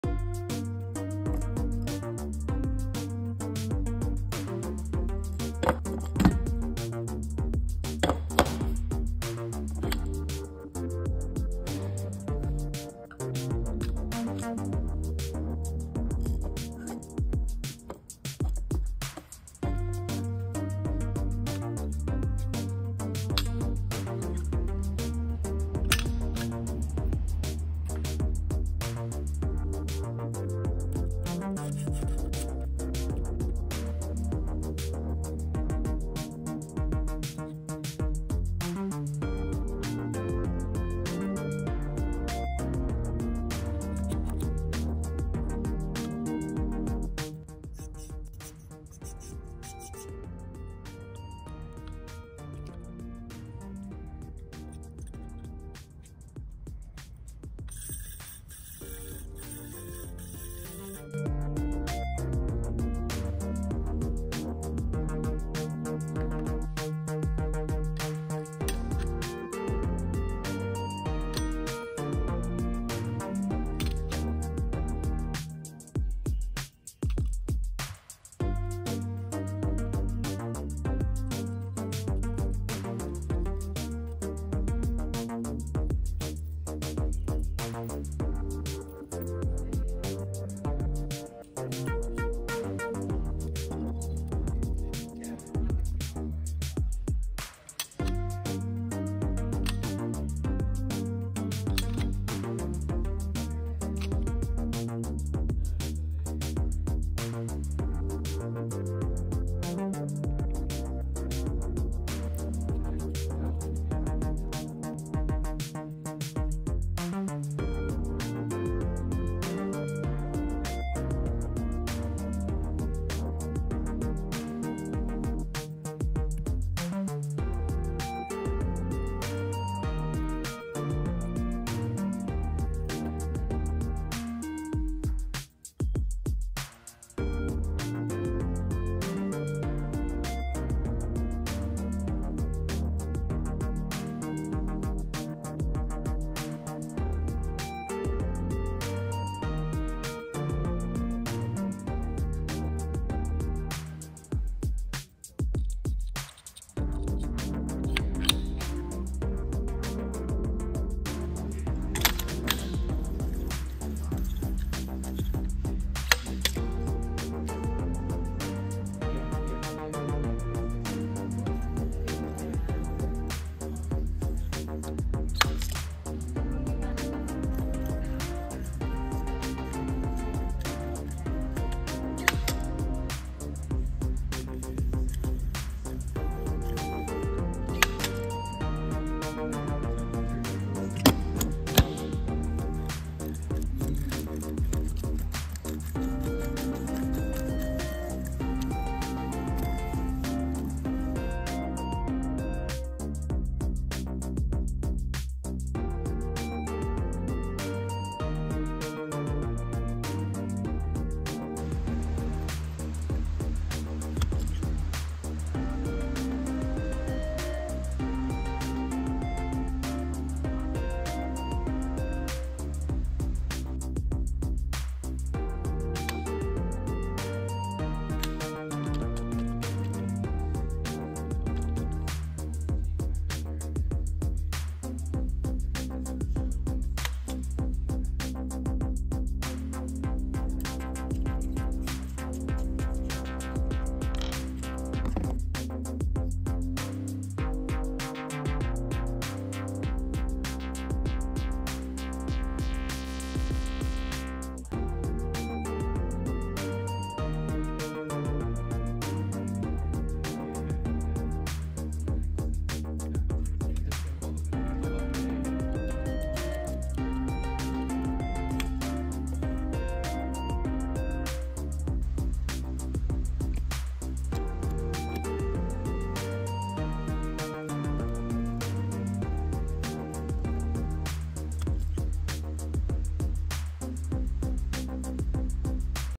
AutoComb Quickstart Guide
Get up to speed in less than 5 minutes.

00:00 Grip
00:24 Height Adjustment
01:03 Portafilter Lugs
01:44 Replacing Needles
02:39 How to Spin
03:59 How to Clean Needles